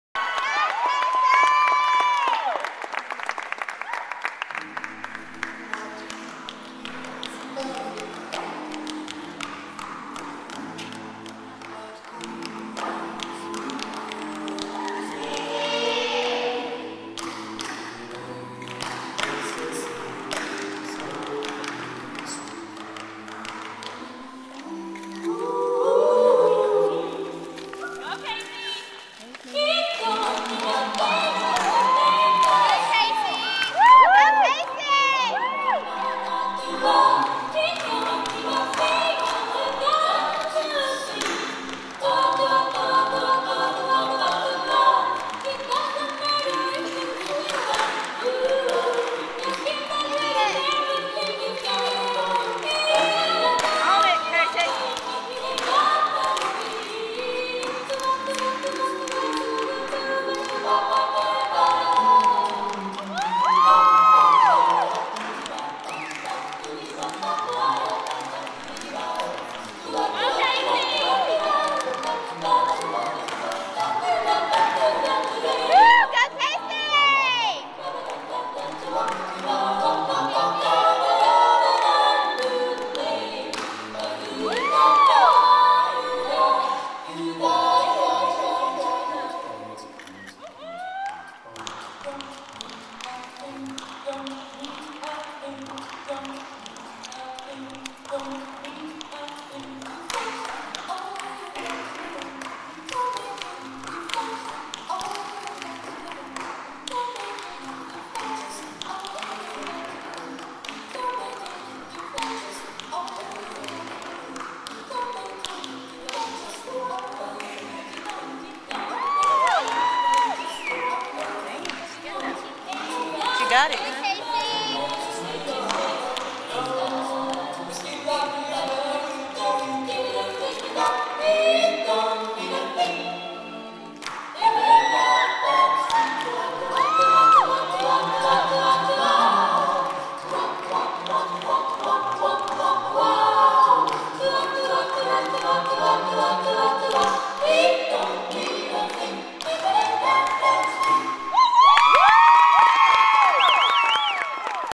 Kacee Crone Tap Solo 2010
Kacee performing her tap solo at Competition in San Antonio.  Choreographed by Katelyn Thompson.